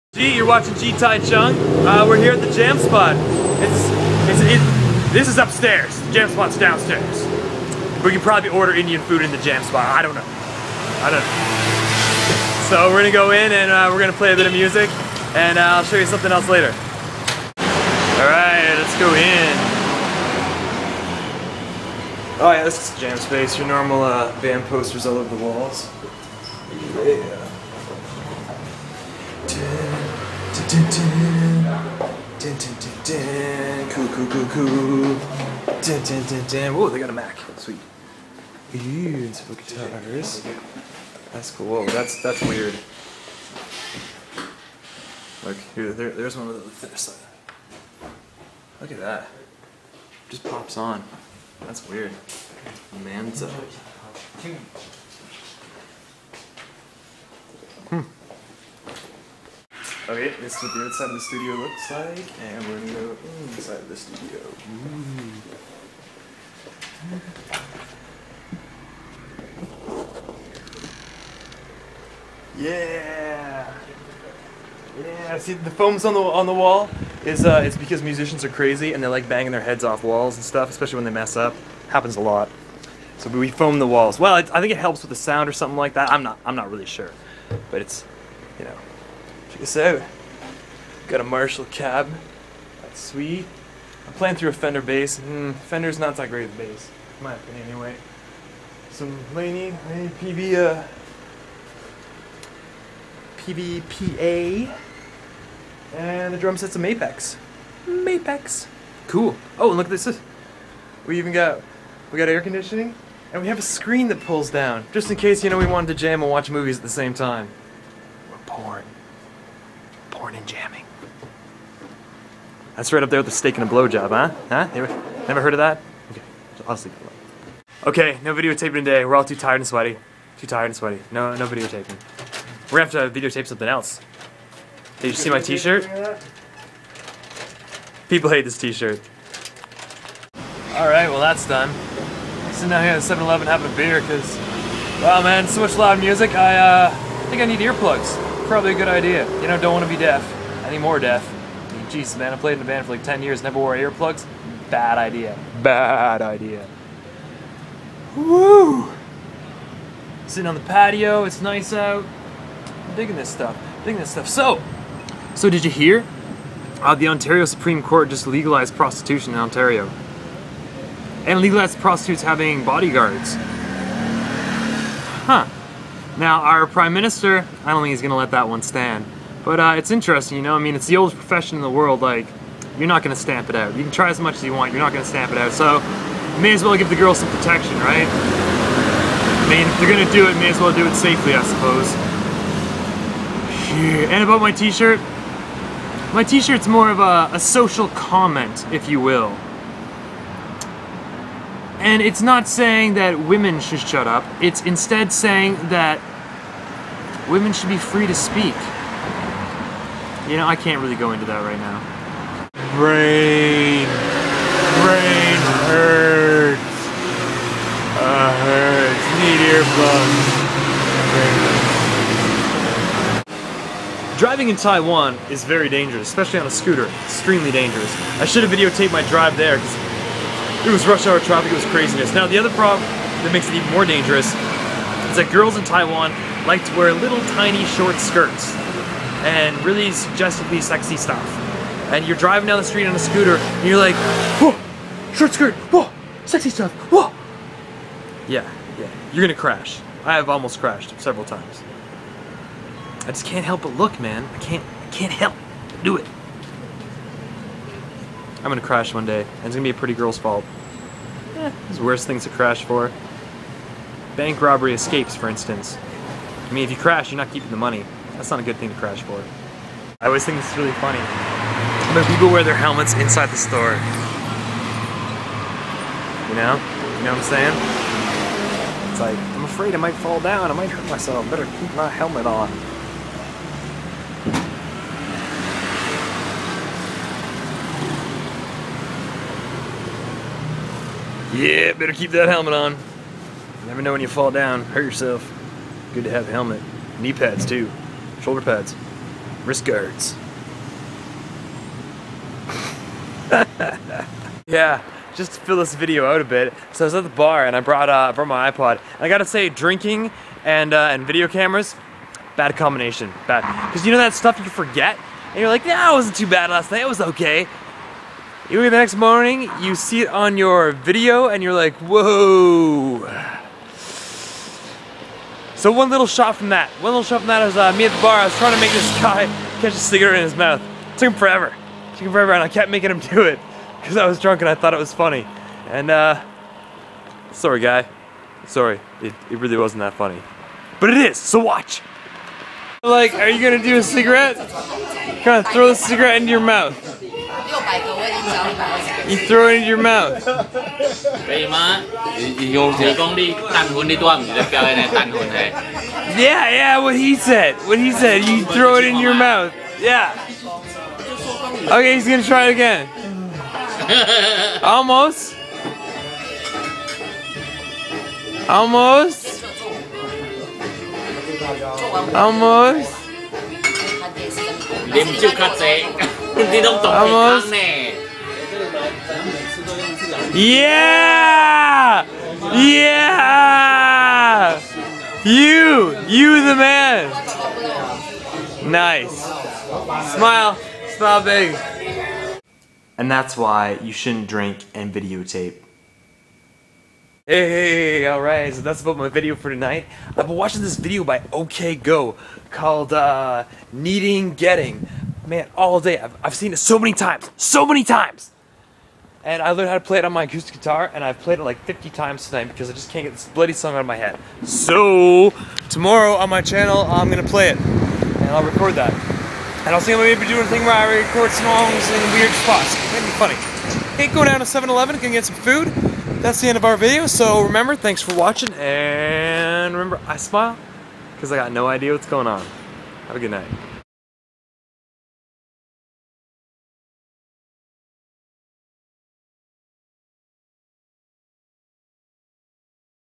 Taiwan- Jan Room, Weird Guitar, Cigarette Trick
A shot of the room we jam in, a weird guitar outside and a drunk dude tryin to catch a cigarette in his mouth!